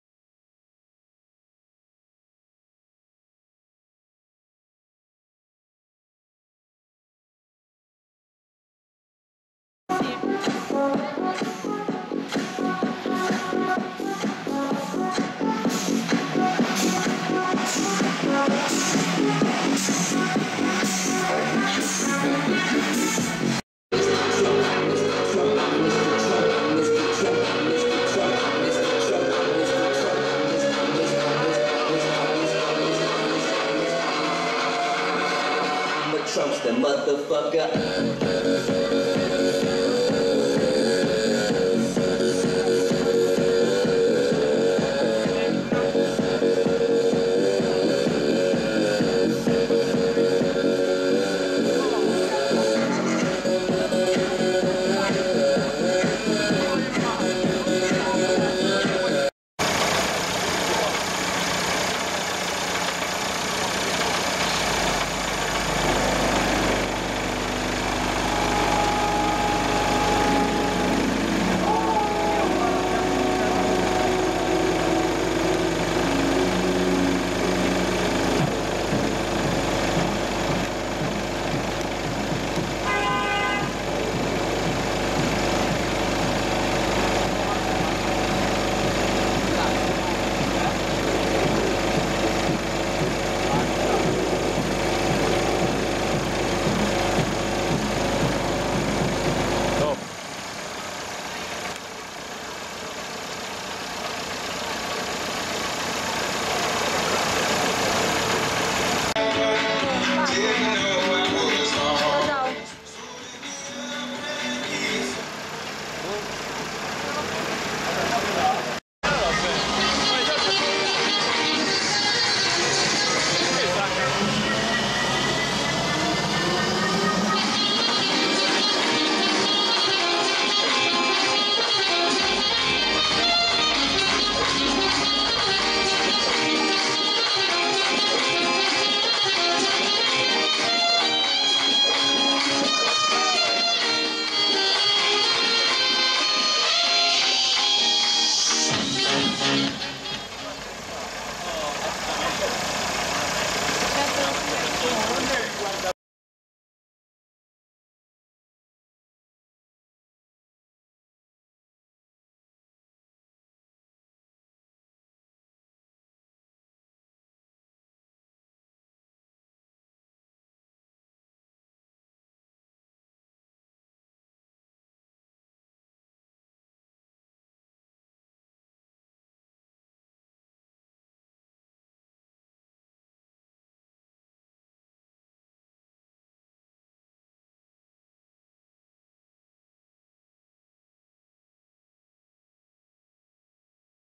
DEMOCAR ZACKS RACE by Dh Sound and Dado Tuning le misurazioni trofeo Shark
La Wolf di Debora Wolf (facebook) monta questi componenti 8-medi alta efficienza Zacks CMR200.3.1 + 4-tweeter a compressiopne alta efficienza Zacks PTW445S + 1-Sub-Woofer Zacks Race 100.15 il tutto amplificato con 2-Stek K204 in mono sui medi e 1-Steg K401 a ponte 2 canali sui tweeter + 1-Steg K5000 sul Sub-Woofer